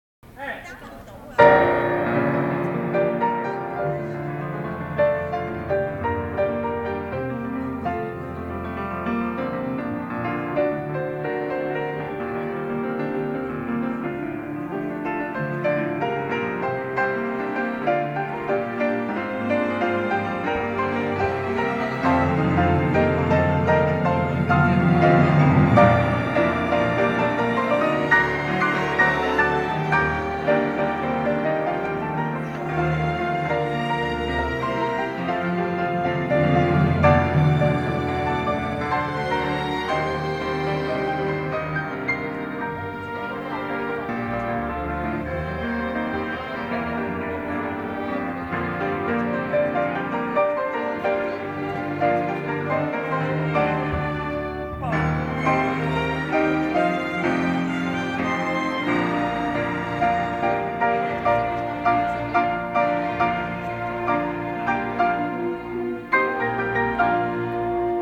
Prof. Gabriel Leung was conducting
Prof. Gabriel Leung was conducting during the orchestra practice of the Medical field 60th National Anniversay Celebration dinner